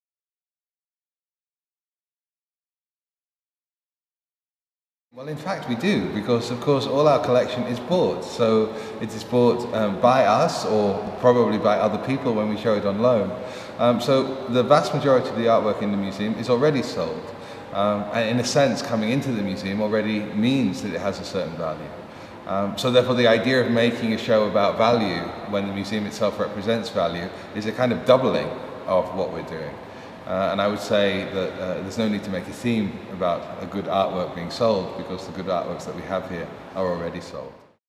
Why do you never discuss the many realities in art?
Director Charles Esche answers questions on the 18 month programme Play Van Abbe in the Van Abbemuseum.
Play Van Abbe consists of 4 parts: part 4 is called "The Pilgrim, the Tourist, the Flaneur (and the Worker)" and takes place from Saturday February 26 2011 until the end of September 2011.
Directeur Charles Esche beantwoordt vragen over het 18 maanden durende programma Play Van Abbe in het Van Abbemuseum.
Play Van Abbe bestaat uit 4 delen: deel 3 heet "De principes van verzamelen - Het verzamelen van principes" en loopt van zaterdag 25 september 2010 tot eind januari 2011.